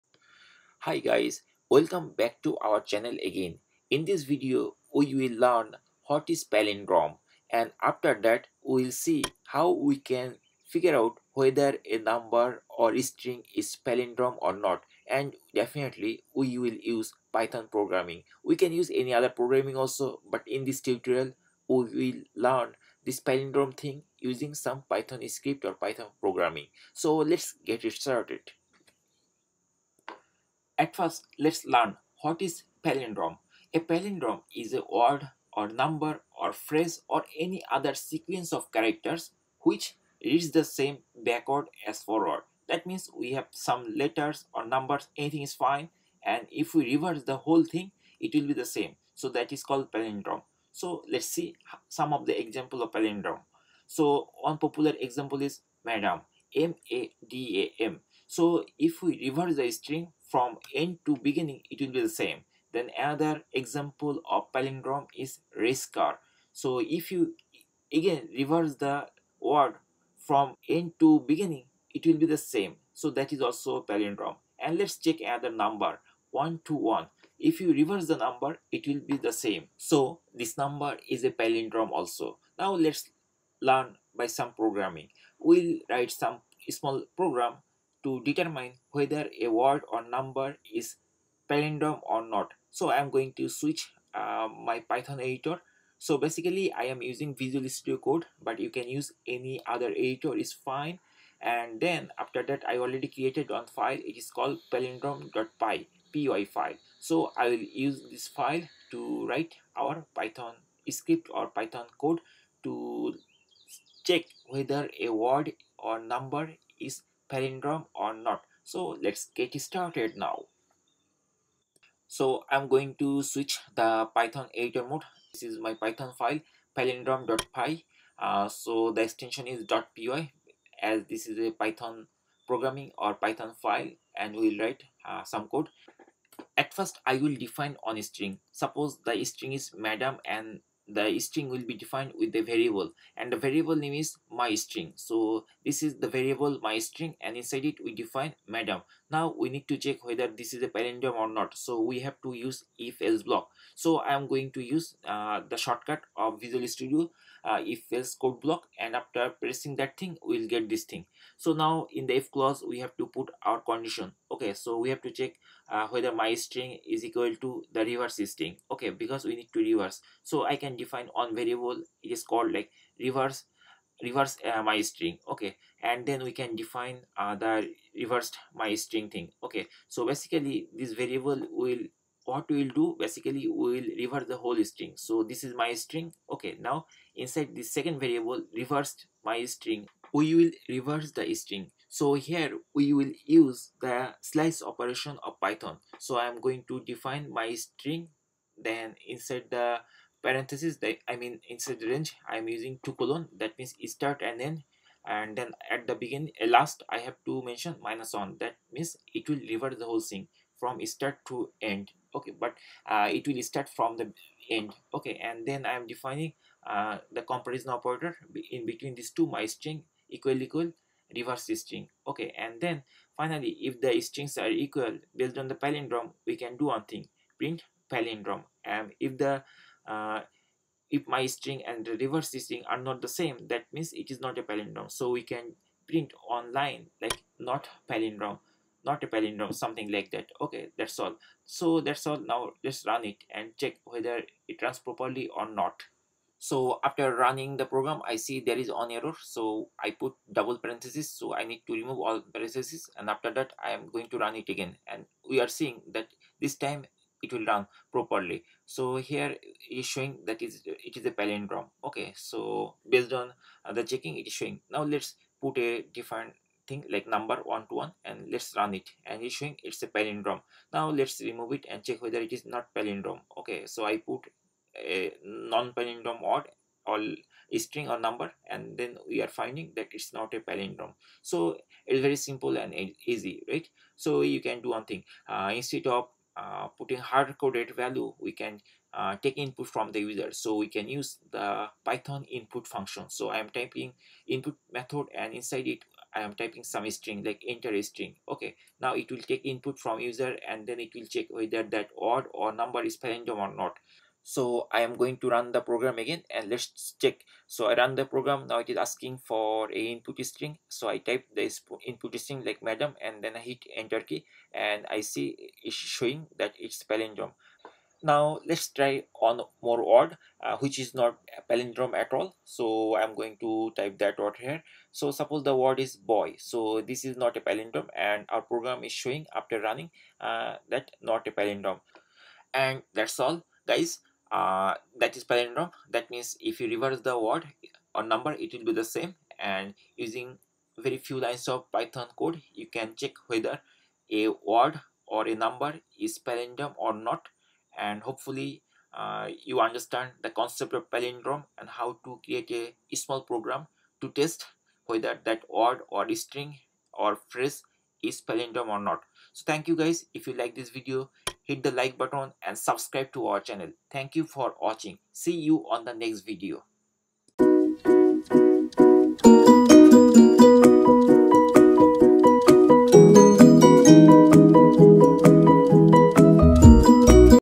Palindrome Python: How to check a number or string is palindrome | Reverse The String And Number
In this Python Tutorials you will learn how to write a Palindrome Program. Using this techniques you can reverse The String And Number. We will use some Advanced Slicing technique to make it very easier to write a python program to check Palindrome or Not Palindrome.